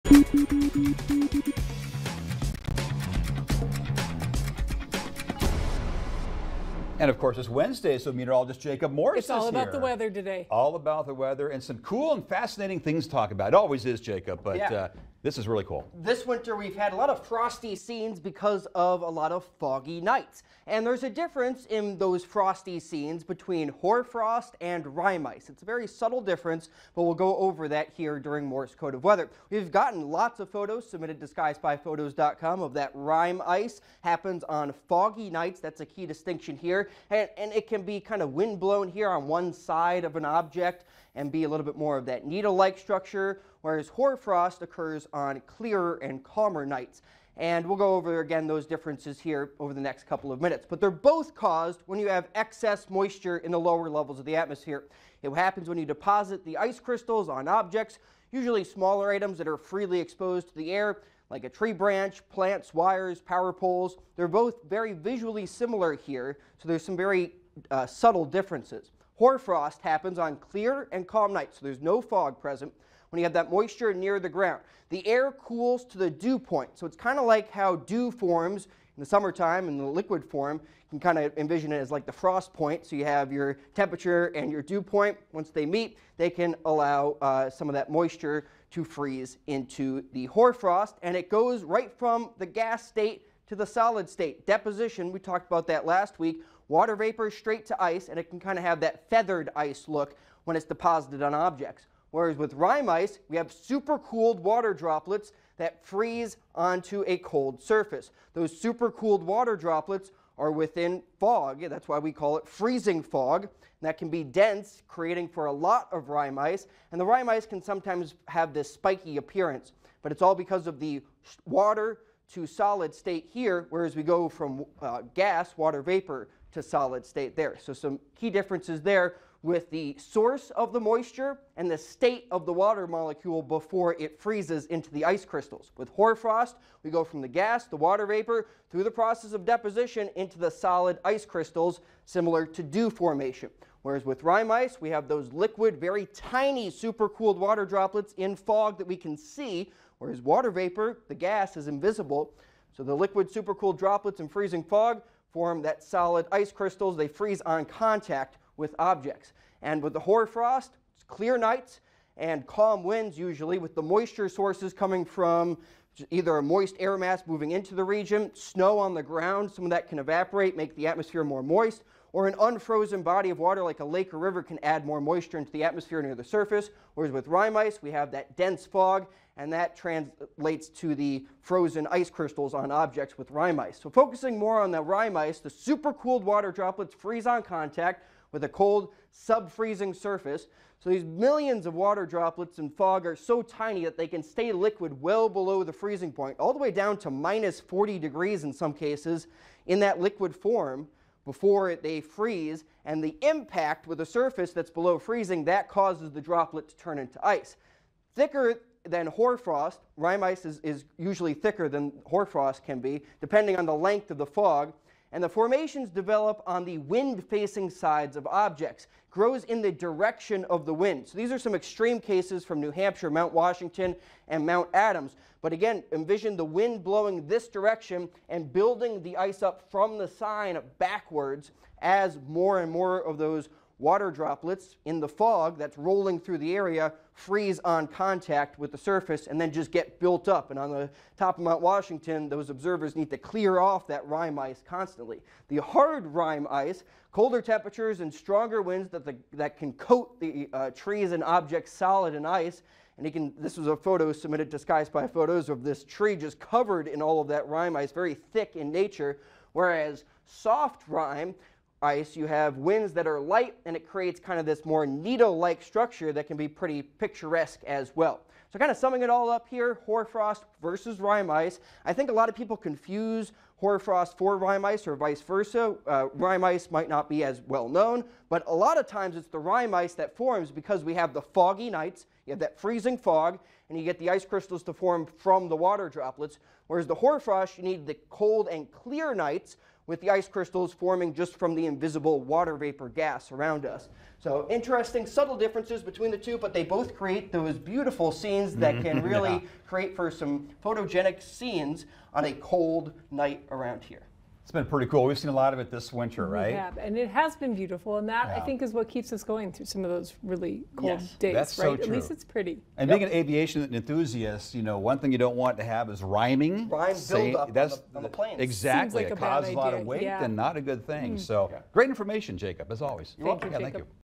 Morse Code of WX: hoar frost vs. rime ice – both create frosty, wintry scenes
We've seen a lot of frosty scenes this winter thanks to hoar frost and rime ice. There are some subtle differences between the two and I explain the science behind them in this week's Morse Code of Weather!